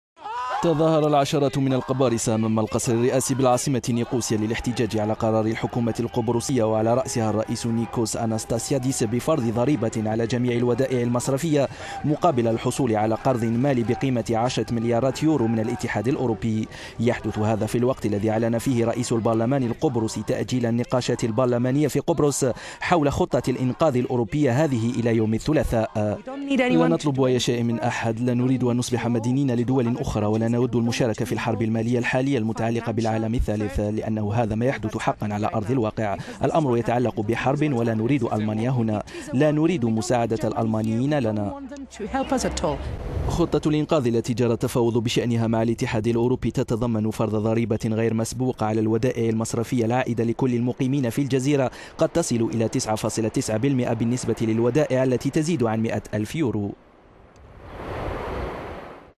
تأجيل جلسة البرلمان القبرصي حول خطة الإنقاذ...
تظاهر العشرات من القبارصة أمام القصر الرئاسي بالعاصمة نيقوسيا للاحتجاج على قرار الحكومة القبرصية وعلى رأسها الرئيس نيكوس اناستاسيادس بفرض ضريبة على جميع الودائع المصرفية مقابل الحصول على قرض مالي بقيمة عشرة مليارات يورو من الاتحاد الأوروبي. يحدث هذا في الوقت الذي اعلن فيه رئيس البرلمان القبرصي تأجيل النقاشات البرلمانية في قبرص حول خطة الإنقاذ الأوروبية هذه إلى يوم الثلاثاء.

احدى المتظاهرات تقول: " لا نطلب أي شيء من أحد، لا نريد أن نصبح مدينين لدول أخرى ولا نود المشاركة في  الحرب المالية  الحالية المتعلقة بالعالم الثالث. لأنه هذا ما يحدث حقا على أرض الواقع. الأمر يتعلق بحرب ولا نريد ألمانيا هنا، لا نريد مساعدة الألمانيين لنا"

خطة الإنقاذ التي جرى التفاوض بشأنها مع الاتحاد الأوربي  تتضمن فرض ضريبة غير مسبوقة على الودائع المصرفية العائدة لكل المقيمين في الجزيرة قد تصل إلى تسعة فاصل تسعة بالمائة بالنسبة للودائع التي تزيد عن مائة ألف يورو  ويتوقع أن تجمع حوالي ستة مليارات يورو بحسب دول مجموعة اليورو.